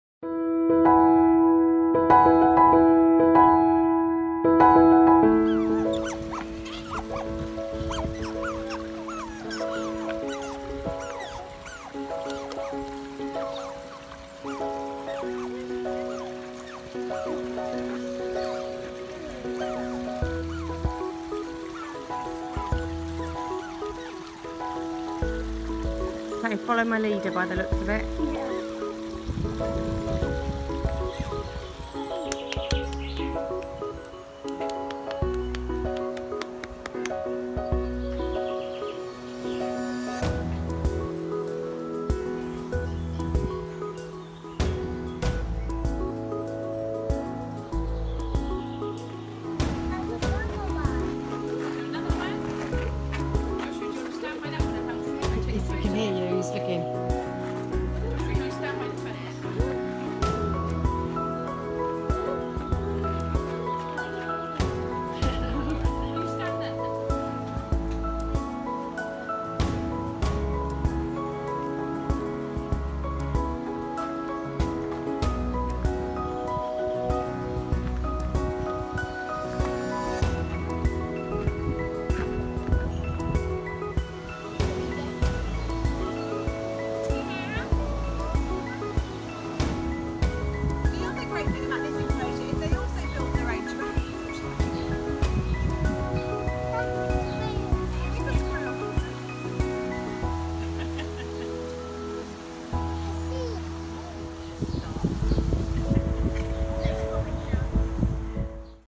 Wildwood Escot
Some of the animals we saw on our visit to Wildwood Escot, including the Red Squirrel.